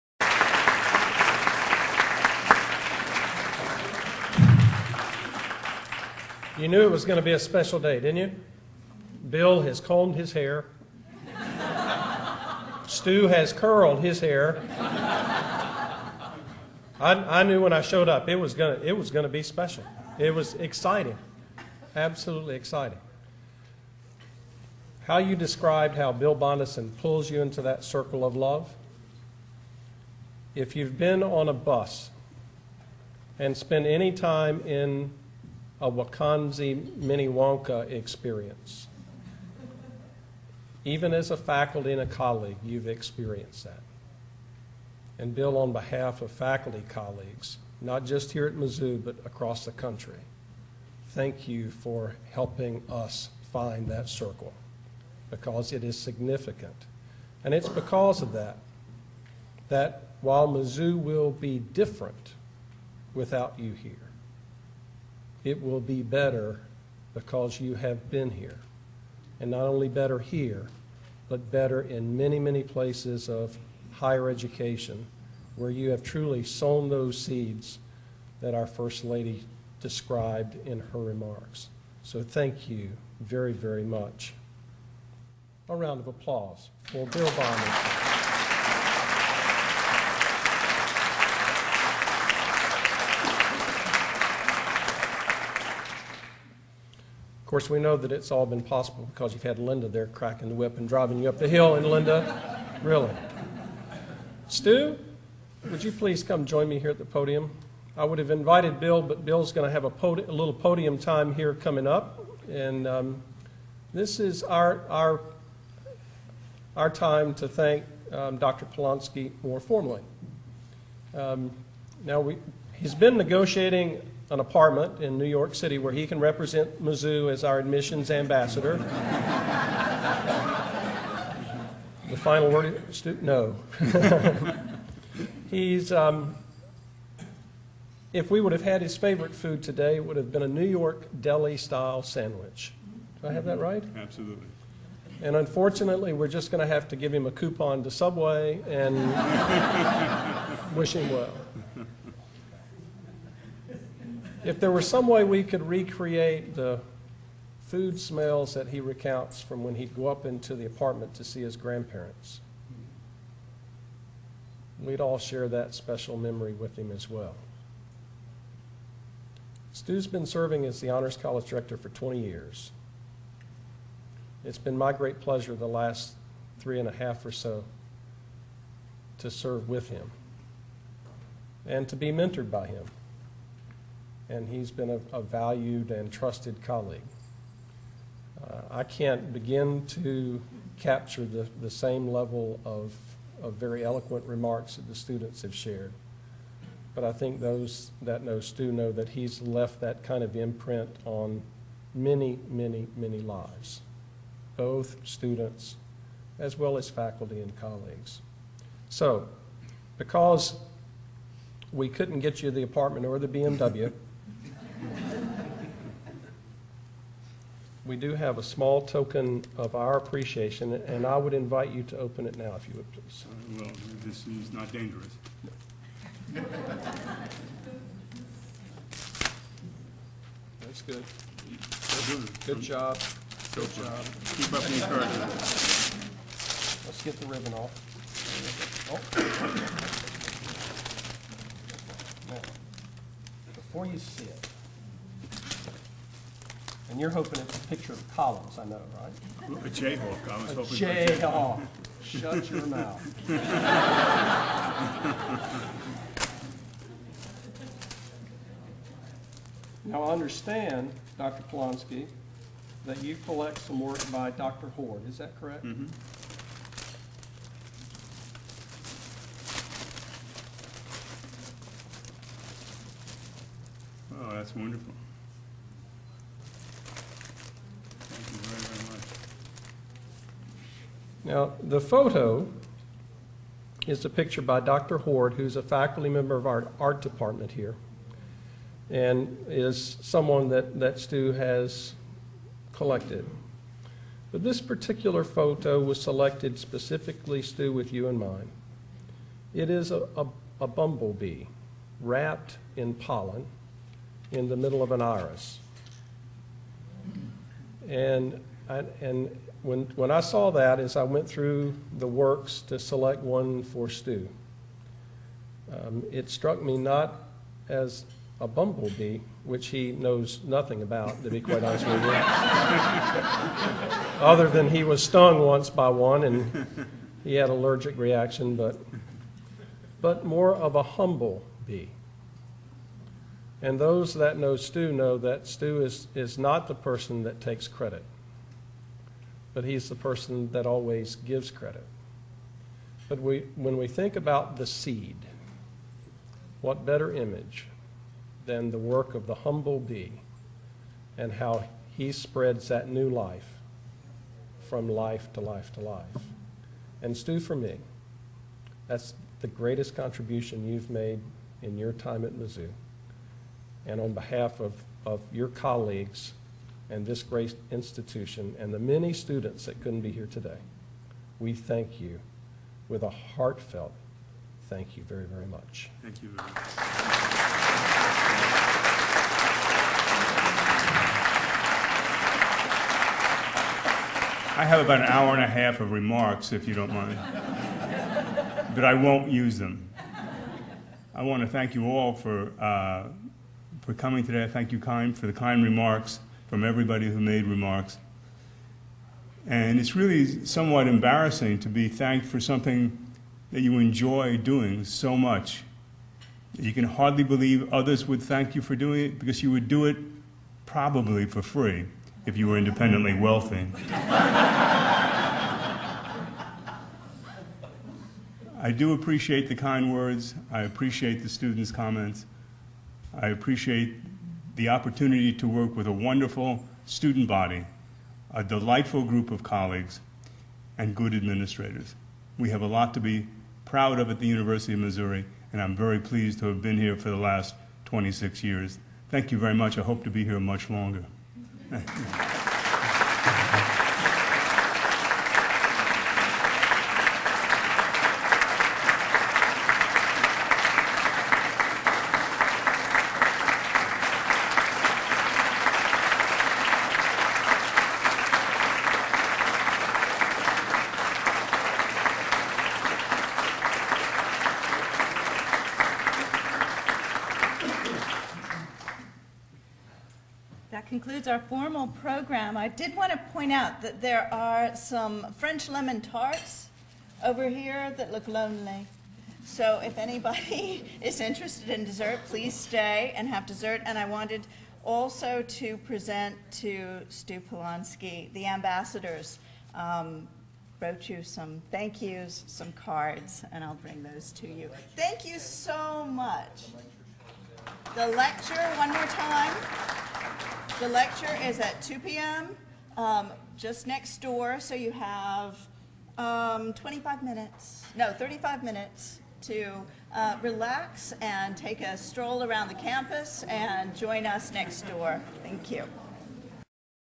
Palonsky Bondeson Luncheon Testimonials Part 4
Testimonials given at a luncheon for Stuart Palonsky and Bill Bondeson on May 7, 2011, at the University of Missouri. Includes testimonial and presentation by MU Vice-Provost for Undergraduate Studies Jim Spain.